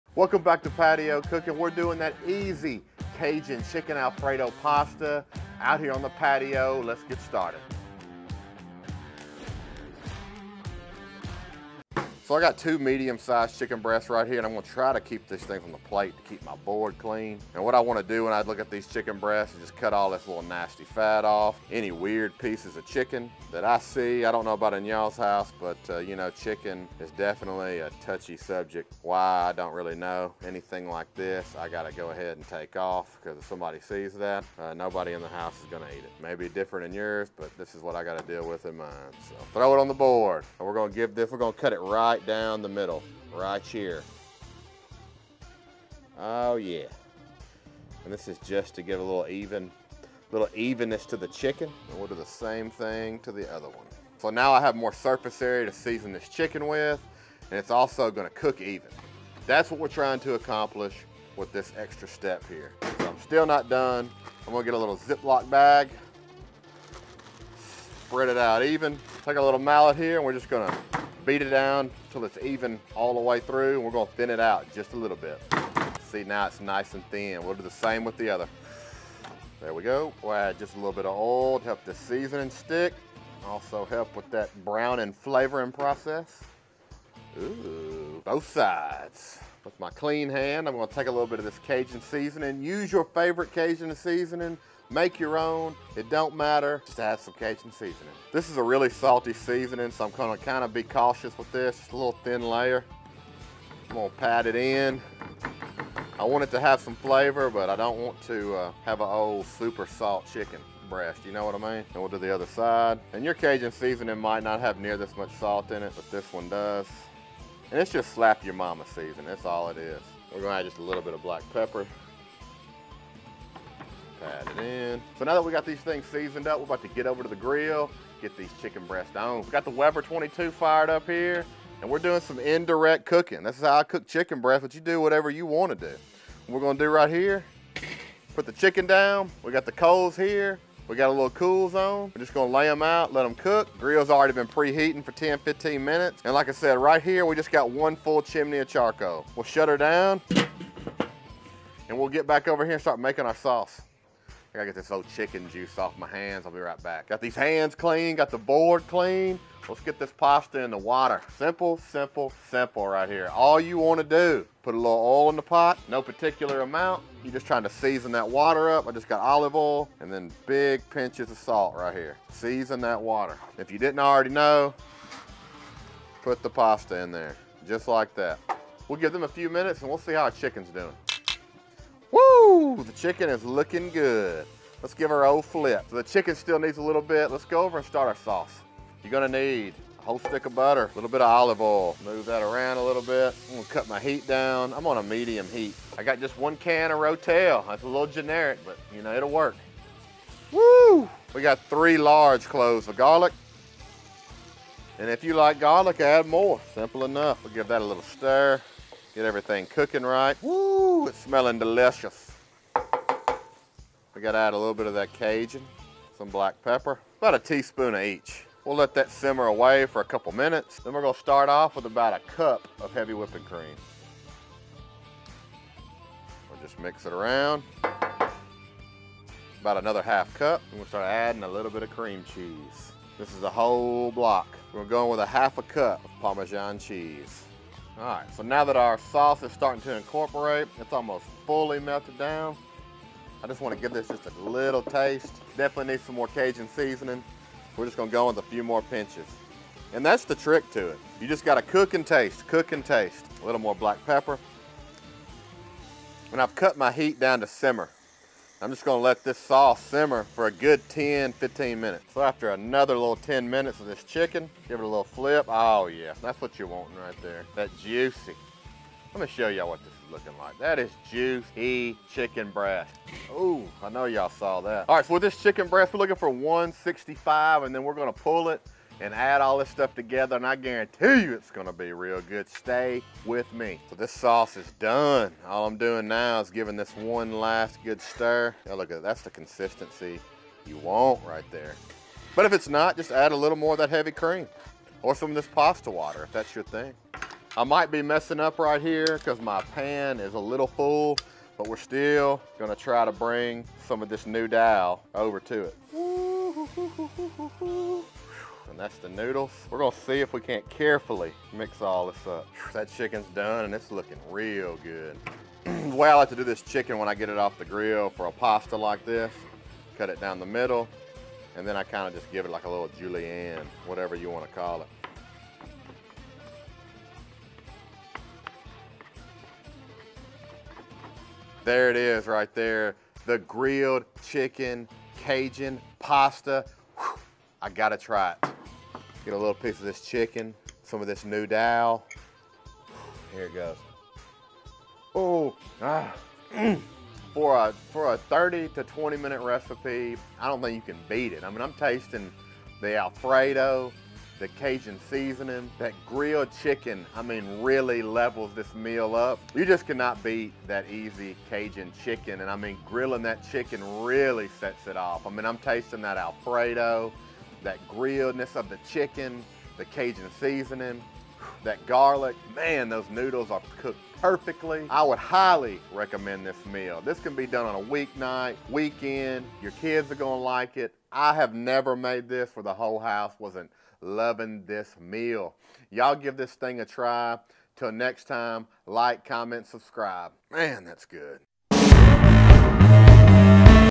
Creamy Cajun Chicken Pasta | Easy Week Night Meal
Ingredients
1.5 - 2 cups
12 oz noodle
1 stick of butter
1 block cream cheese
1 can of rotel
1 tbsp cajun seasoning
2 tsp pepper
1lb chicken  breast